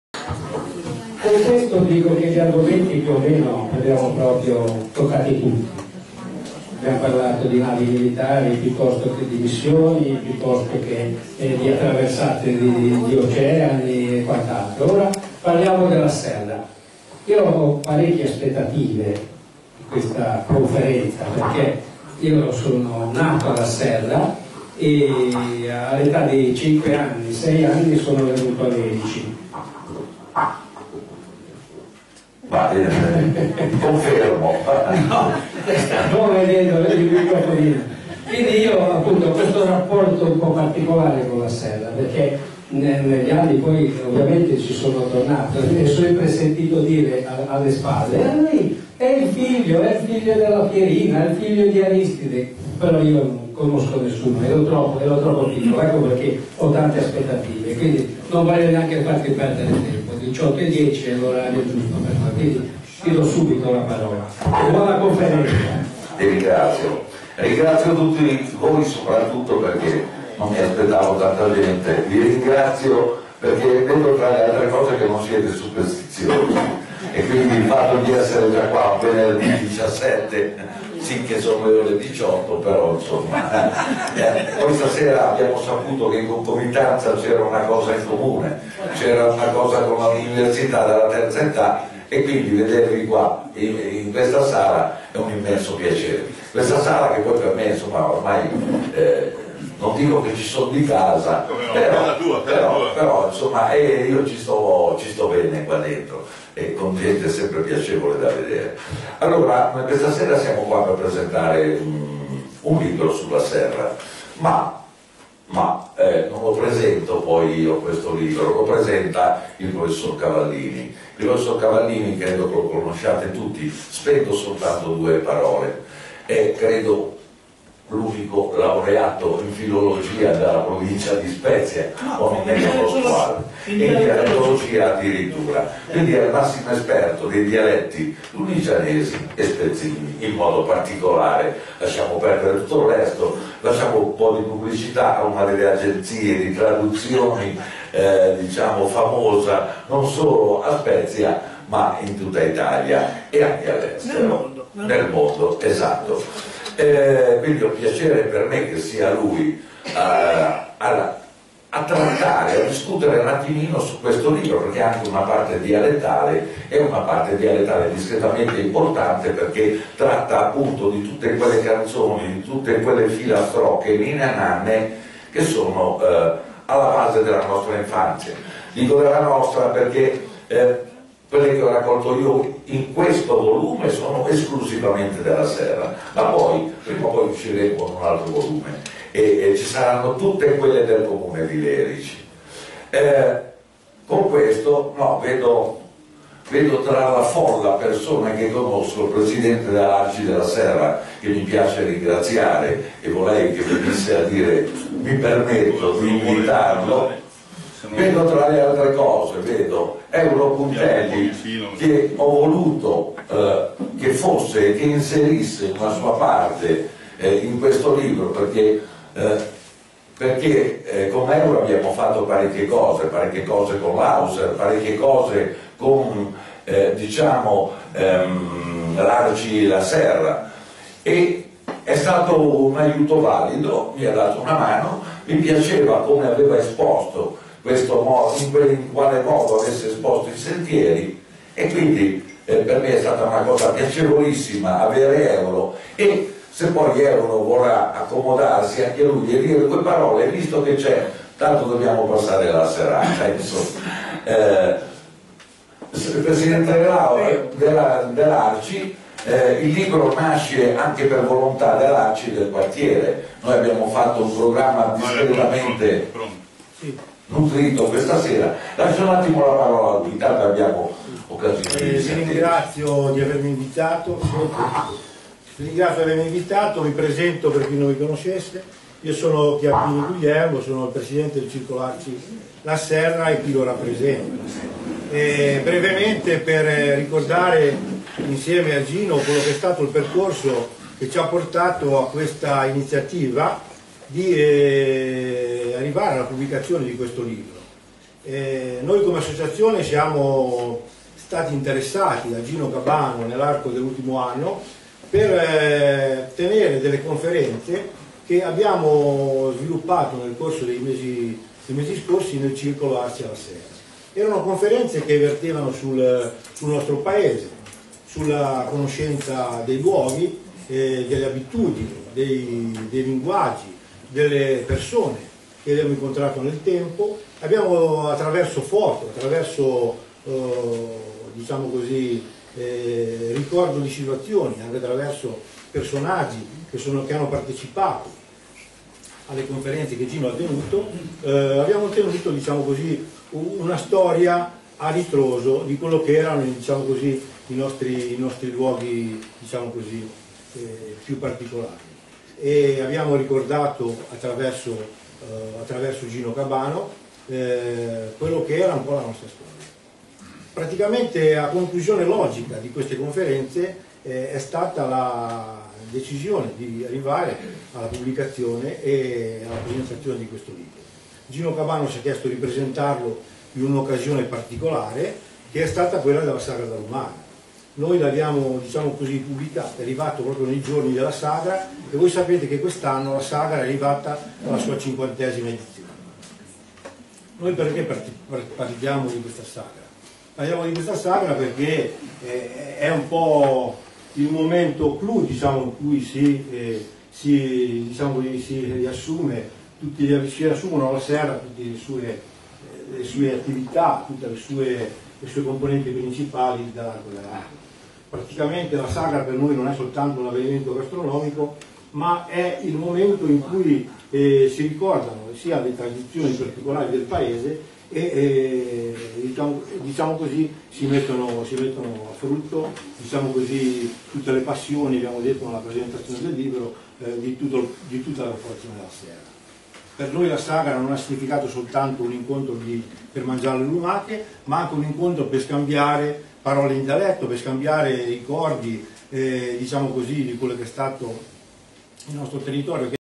introduzione presentazione Serra
Nella sede dei Marinai d'Italia di Lerici la presentazione del libro "Serra  radiografia di un paese" di Gino Cabano venerdì 17 novembre 2017 con il Prof. Pier Giorgio Cavallini